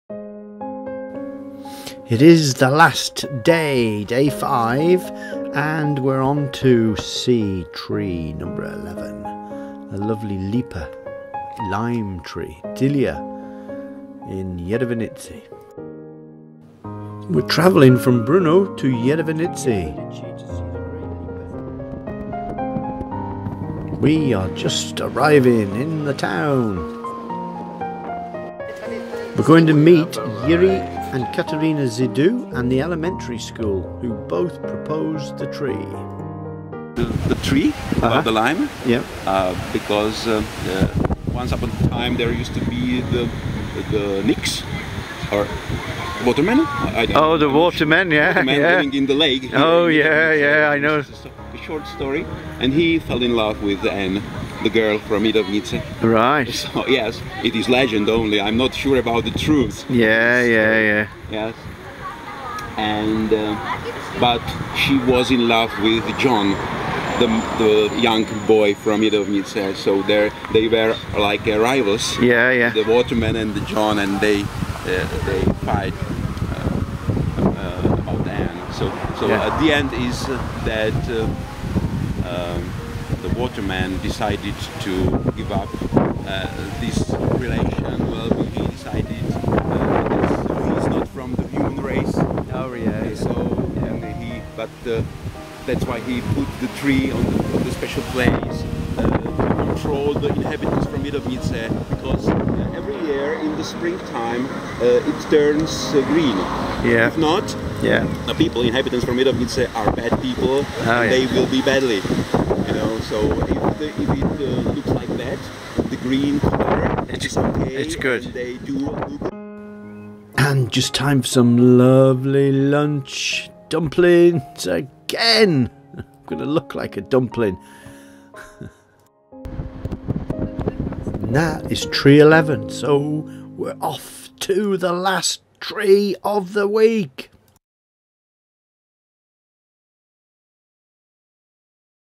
Czech Tree of the Year 2014 Day 5 Tree 11 Jedovnice Lipa
Proposer: Elementary school Jedovnice, SPŠ Jedovnice and Jiří Zidu and Katerina Zidu
Type of tree: small-leaved lime
Age of trees: 300 years
Circumference of the tree in 130 cm: 523 cm
Location: South Moravian Region district Blansko, village Jedovnice street at roughly lípy
Photo: Lenka Grossmannová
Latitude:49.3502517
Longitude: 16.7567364
Story:
Gross, large, lime Jedovnice on the edge looking down into the region for about 300 years. It was planted in the days of Count Karl Ludwig Rogendorf that Jedovnice Baroque mansion built near a village founded Rogendorf. Lipa is just beside the road from the lodge around the church in the new village, near the grave of those who succumbed to cholera. Good Count is long dead, mansion demolished church rebuilt village Rogendorf renamed - just a rough lime remains a fixed point in a changing world, today, and St. James pilgrimage route.
Money raised ballot used for the treatment of lime and planting trees in the arboretum at the elementary school Jedovnice.

Navrhovatel: ZŠ Jedovnice, SPŠ Jedovnice a Jiří Židů
Druh stromu: lípa malolistá
Stáří stromu: 300 let
Obvod stromu ve 130 cm: 523 cm
Lokalita: Jihomoravský kraj, okres Blansko, obec Jedovnice, ulice U Hrubé lípy
Foto: Lenka Grossmannová
Zeměpisná šířka: 49,3502517
Zeměpisná délka: 16,7567364
Příběh: Hrubá, tj. velká, lípa na okraji Jedovnic shlíží do kraje už asi 300 let. Byla vysazena za časů hraběte Karla Ludvíka z Rogendorfu, který v Jedovnicích vystavěl barokní zámeček a nedaleko založil ves Rogendorf. Lípa stojí právě u cesty od zámečku kolem kostela do nové vsi, blízko hrobu těch, kdo podlehli nákaze cholery. Dobrý hrabě je dávno po smrti, zámeček zbořen, kostel přestavěn, ves Rogendorf přejmenována -- jen Hrubá lípa zůstává jedním z pevných bodů v měnícím se světě, dnes i na poutní Svatojakubské cestě.
Peníze získané hlasováním použijí k ošetření lípy a výsadbě stromů do arboreta u ZŠ Jedovnice.